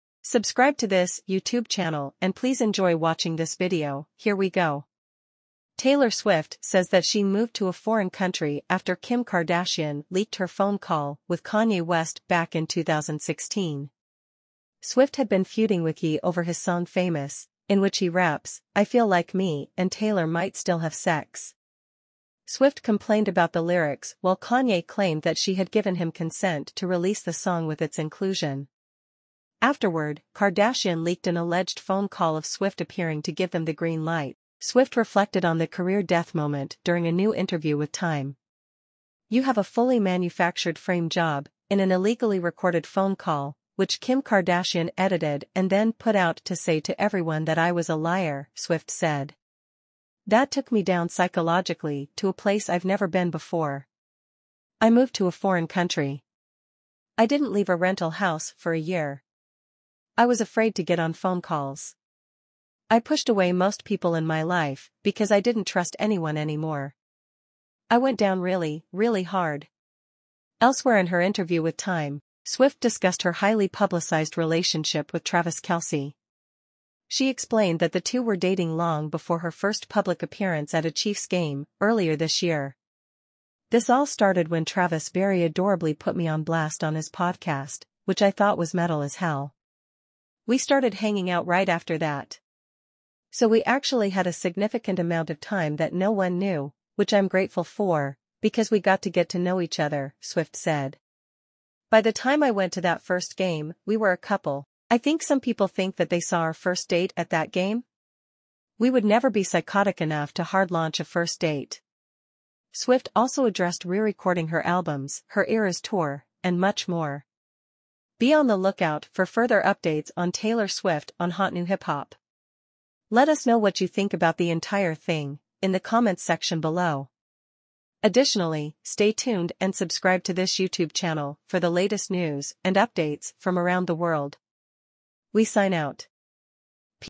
Taylor Swift Reflects On Kanye West & Kim Kardashian's Leaked Phone Call
Taylor Swift says she "went down really, really hard" after her leaked phone call with Kanye West and Kim Kardashian.